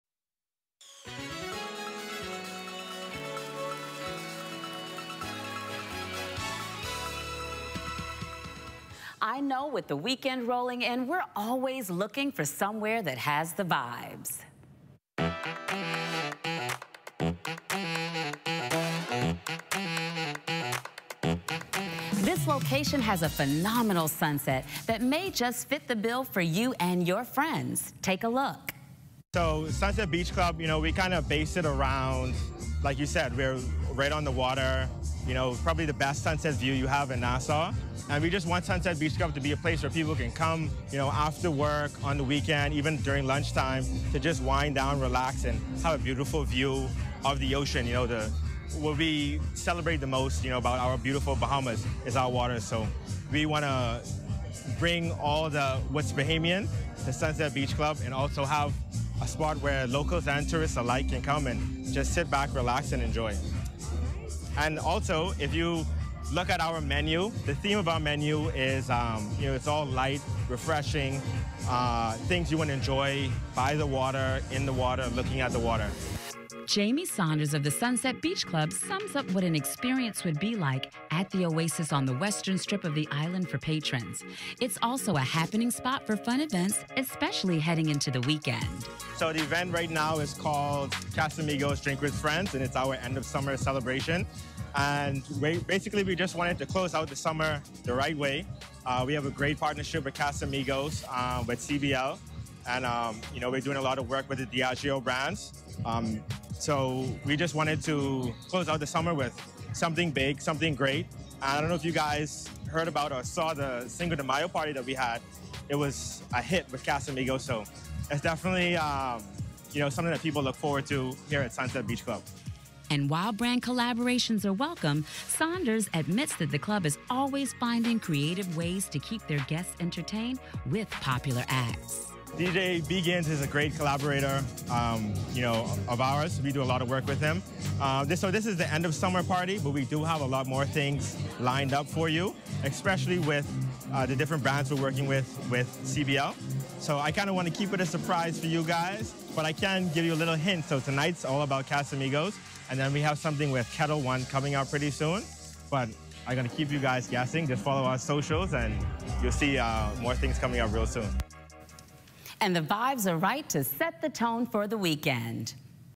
What's Trending: Weekend Vibes
Sunset Beach Club General Manager, Jamie Saunders tells when you should make this place a part of your weekend plans.